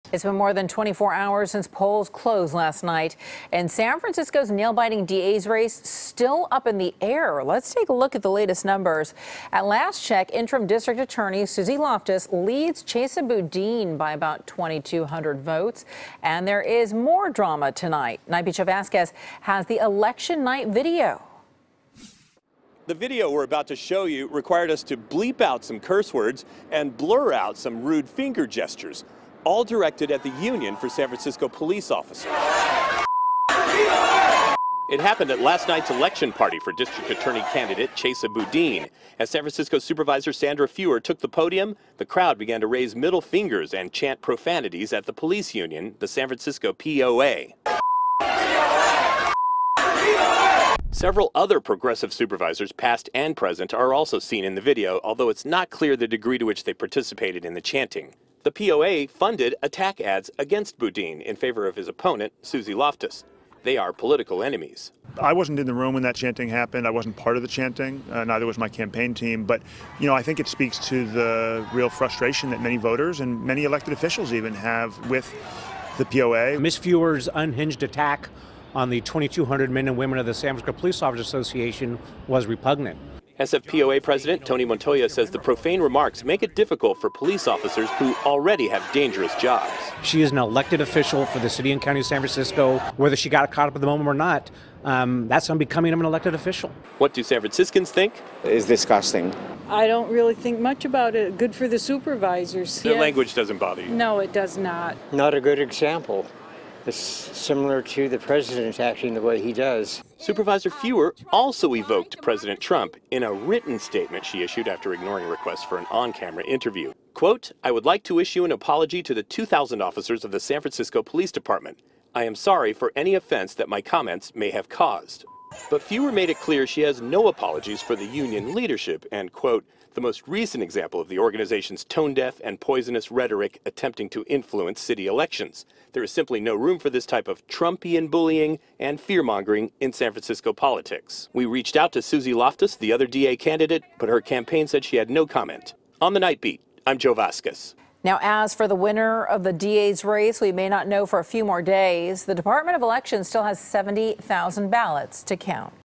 SF Supervisor At Podium As Crowd Chants 'F*** The POA' At Election Party
At an election party for San Francisco DA candidate Chesa Boudin, the crowd began to raise middle fingers and chant profanities at the city's police union, the POA. Standing at the podium was Supervisor Sandra Fewer, among others. Joe Vazquez reports. (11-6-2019)